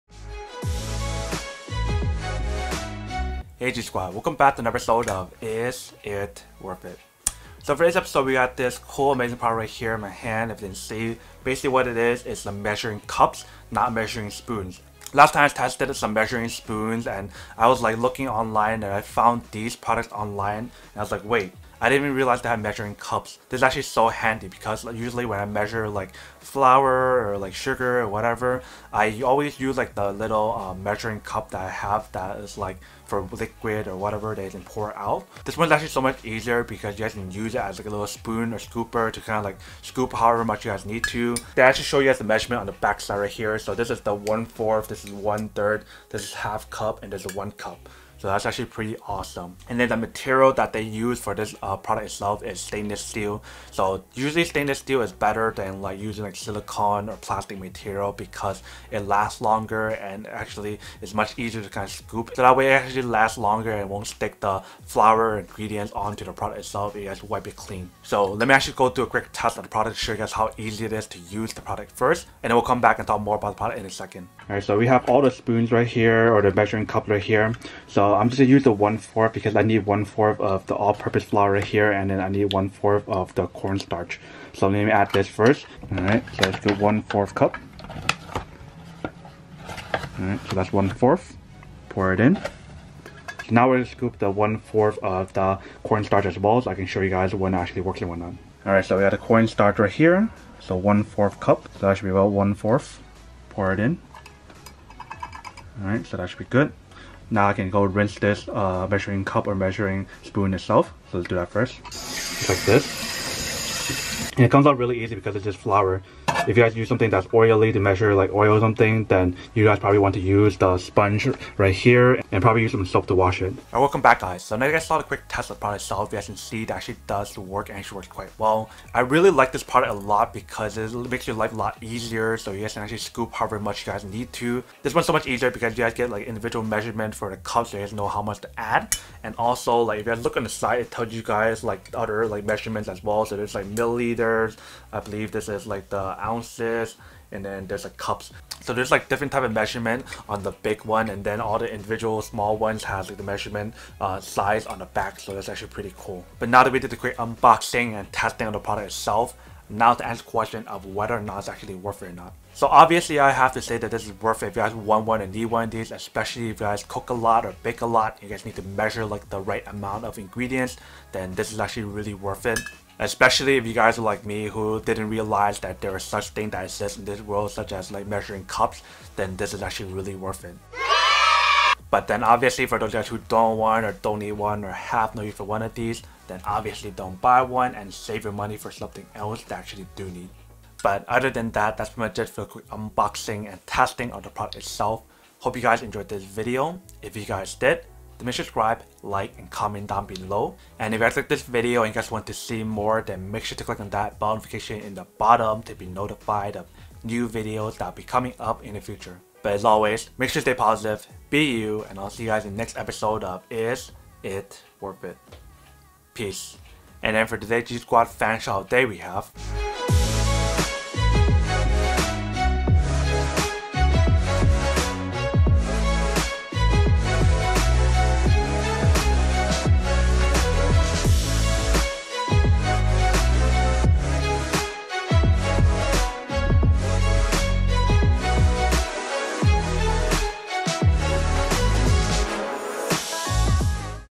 Stainless Steel Measuring Cups Review! Worth it?
Are you in search for the best & affordable stainless steel measuring cup to measure the correct amount of ingredients for cooking or baking and etc? Check out the video to find out if this product is for you!

If you want:
Stainless Steel Measuring Cups

Cuisinart Stainless Steel Measuring Cups